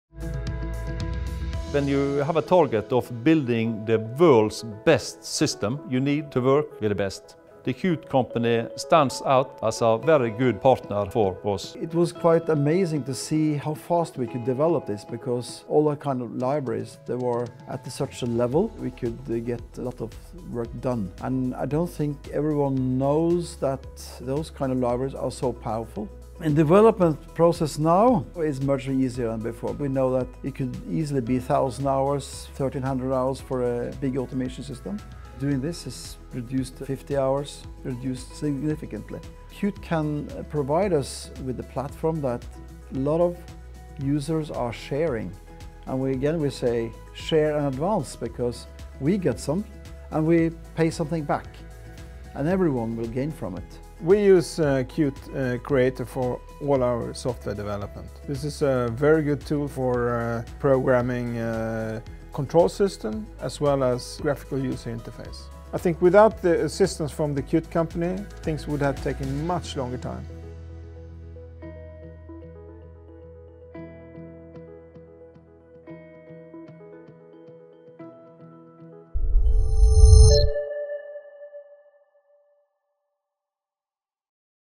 Qt brings Ulstein’s daring & industry disruptive vision to reality
The first project with Qt and The Qt Company services team, has enabled Ulstein Power & Control AS to prepare ship building for a digital future with a wave breaking automation system. Ulstein’s solution is a flexible, modular and user-friendly automation system providing a wide range of functionality and applications. The solution gives integrated control and monitoring of signal and alarm monitoring system and information systems; engines, propulsion and thrusters, vessel performance; power management systems (PMS), and vessel’s power plant and power distribution systems.